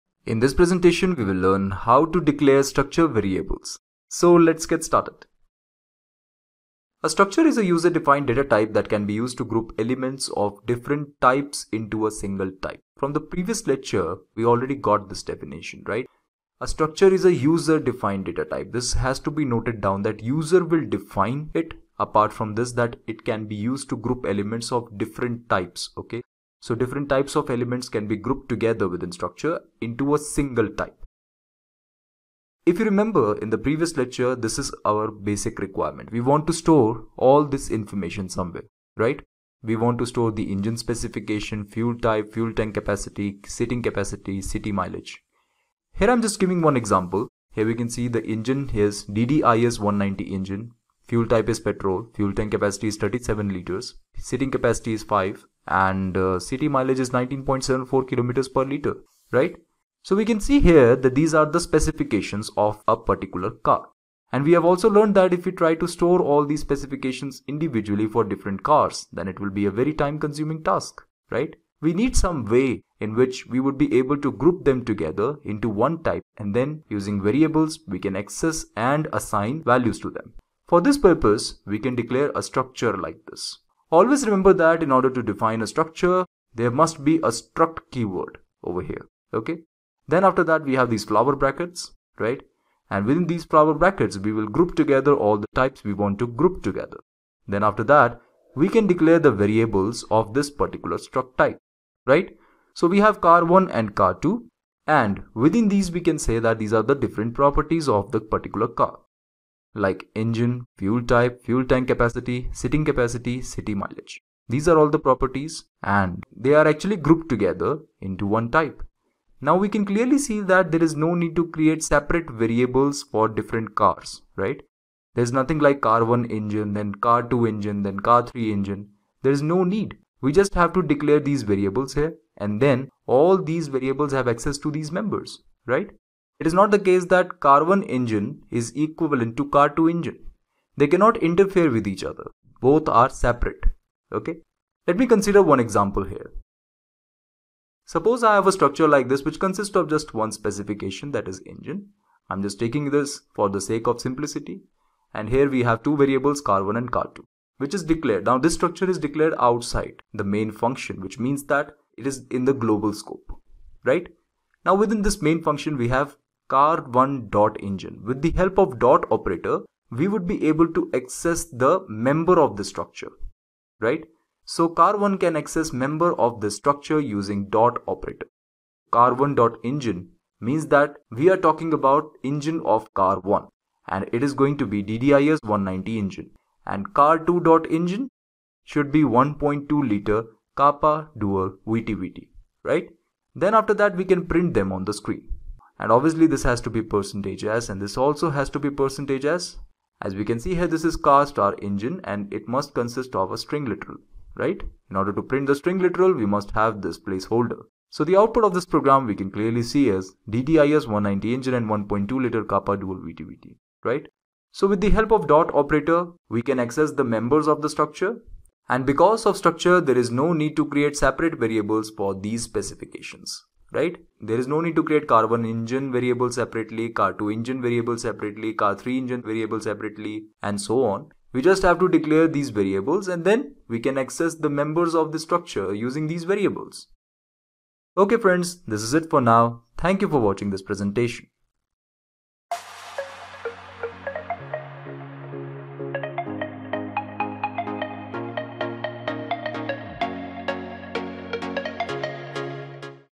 Declaring Structure Variables
C Programming: Declaring Structure Variables in C Programming.
Topics discussed:
1) The definition of Structures in C Programming.
2) Declaration of Structure Variables.